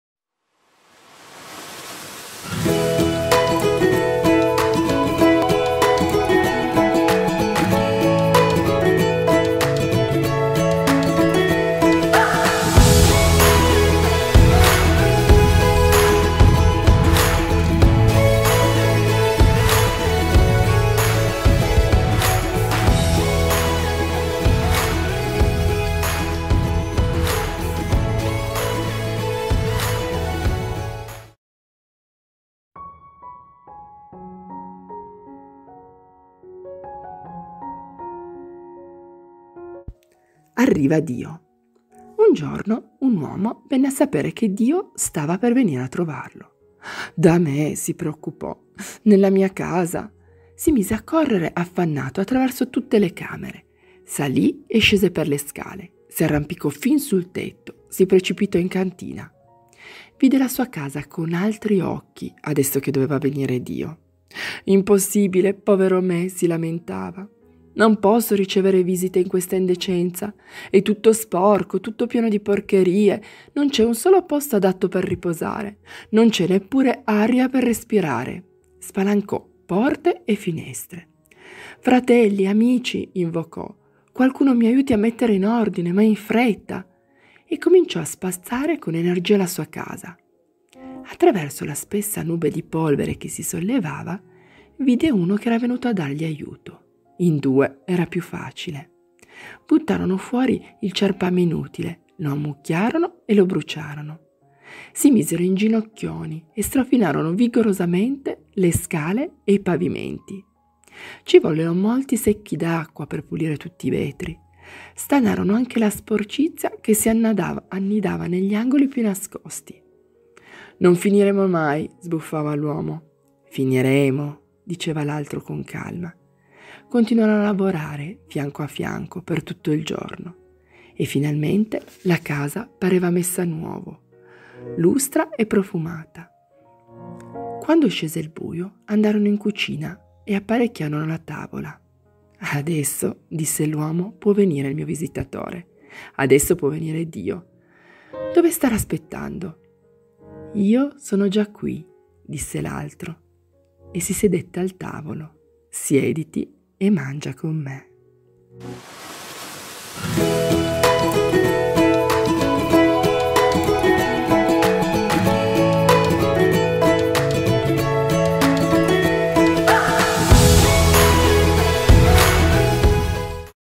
LA SANTA MESSA DA SCOPRIRE INSIEME! - Prima Storia: ARRIVA DIO!
La storia che viene raccontata in questo video fa parte di un cammino in 4 tappe per conoscere meglio la struttura ed i significati del rito della Messa.
Tratta da "Altre storie" di Bruno Ferrero  (Ed. Elledici)